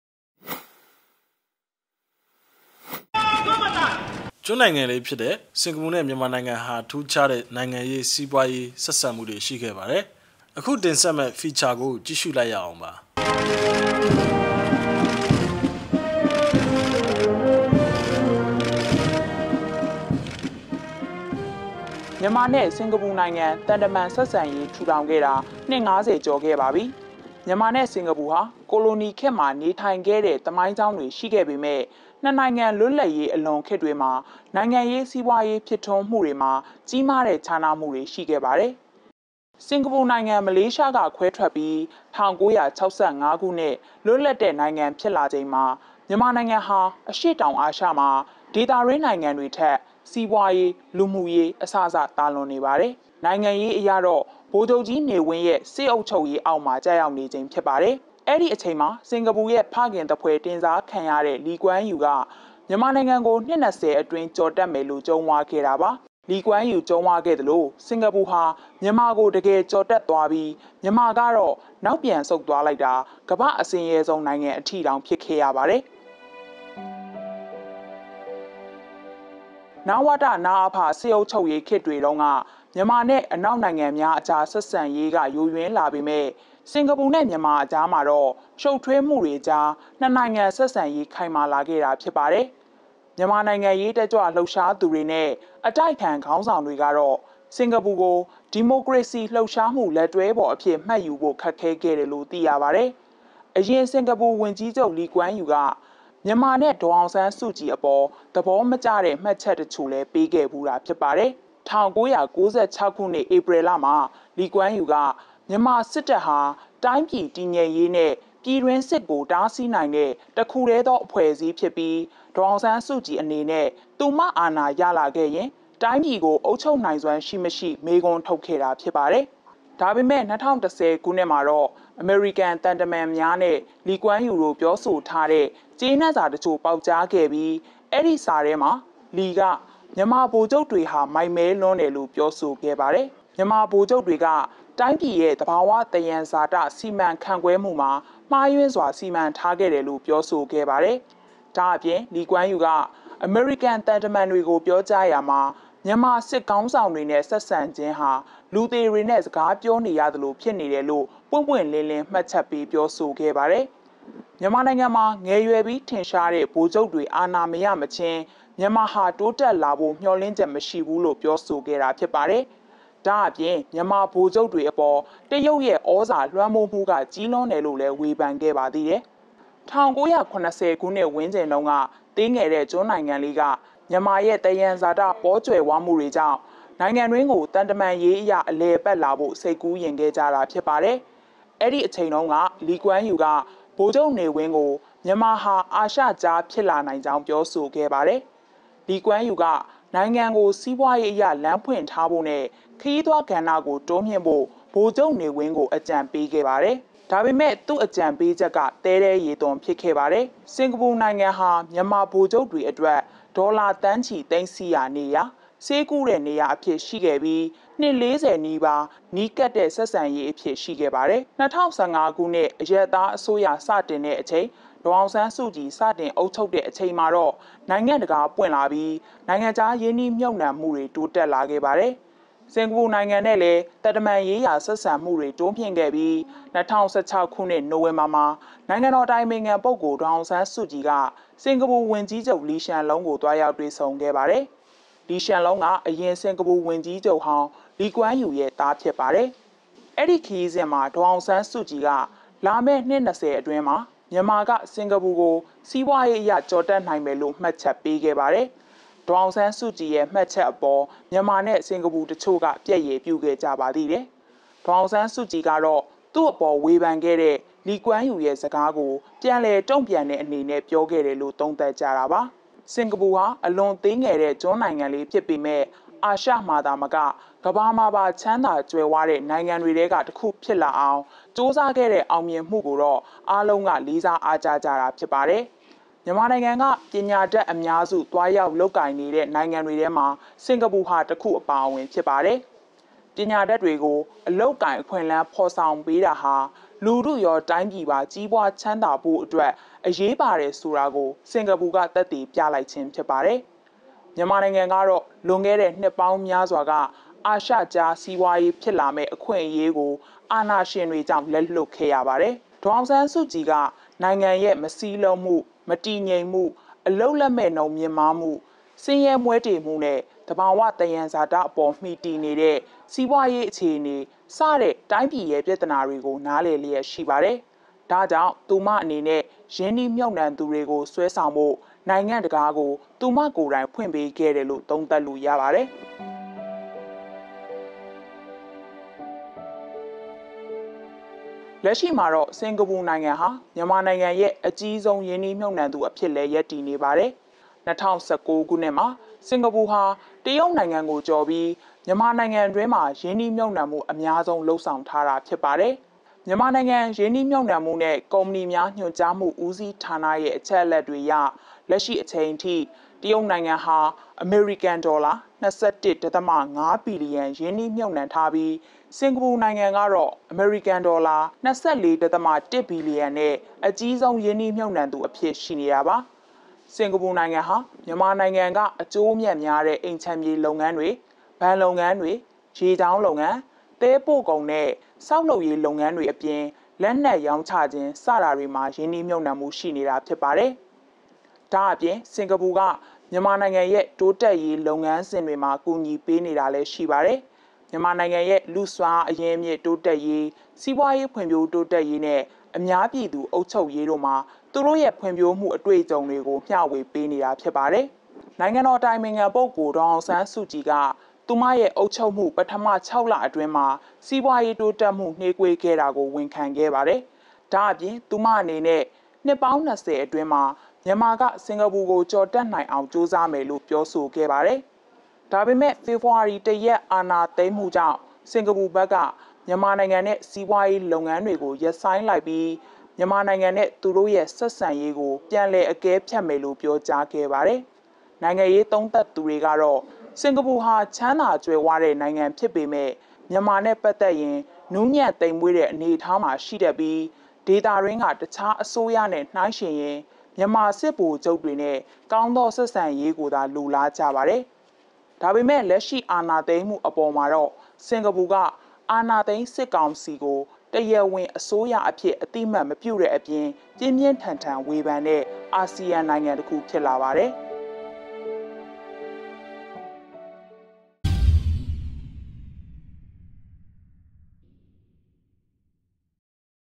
မြန်မာနှင့် စင်္ကာပူ
ကျွန်းနိုင်ငံလေးဖြစ်တဲ့ စင်္ကာပူနဲ့ မြန်မာနိုင်ငံဟာ ထူးခြားတဲ့ နိုင်ငံရေး စီးပွားရေး ဆက်ဆံမှုတွေ ရှိခဲ့ပါတယ်။ အခုတင်ဆက်မဲ့ feature မှာ ကြည့်ရှုနိုင်ပါတယ်။

       shorturl.at/lFPQ0